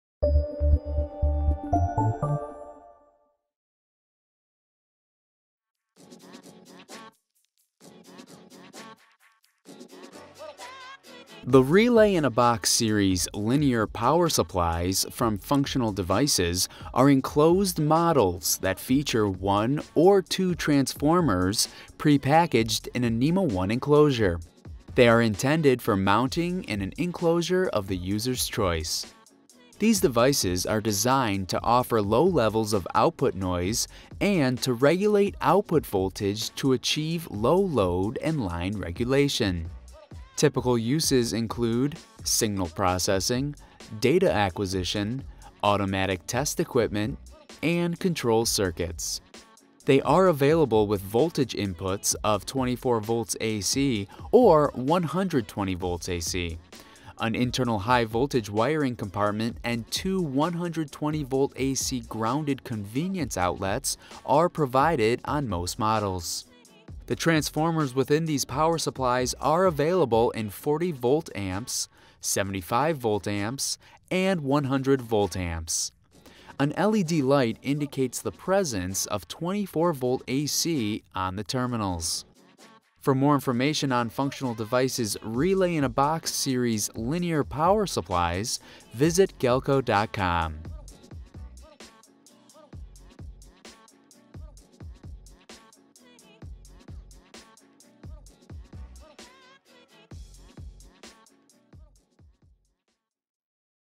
Functional Devices RIB Series, Linear Power Supply
The Relay in a Box series linear power supplies from Functional Devices are enclosed models that feature one or two transformers pre-packaged in a NEMA 1 enclosure. They are intended for mounting in the enclosure of the user’s choice. These devices are designed to offer low levels of output noise and to regulate output voltage to achieve low load and line regulation. Typical uses include; signal processing, data acquisition, automatic test equipment, and control circuits.